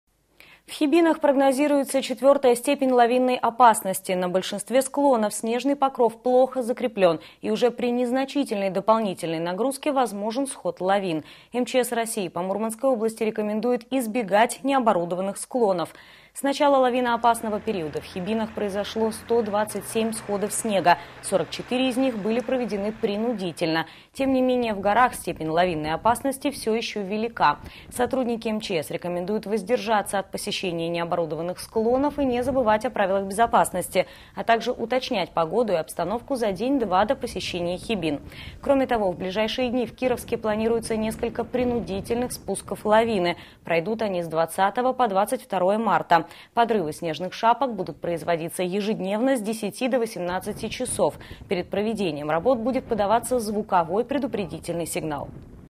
В Хибинах прогнозируется 4 степень лавинной опасности
С начала лавиноопасного периода в Хибинах произошло 127 сходов снега. 44 из них были проведены принудительно. Тем не менее, в облюбованных лыжниками и сноубордистами горах степень лавинной опасности всё ещё велика. Сотрудники МЧС рекомендуют воздержаться от посещения необорудованных склонов и не забывать о правилах безопасности, а также уточнять погоду и обстановку за день-два до посещения Хибин.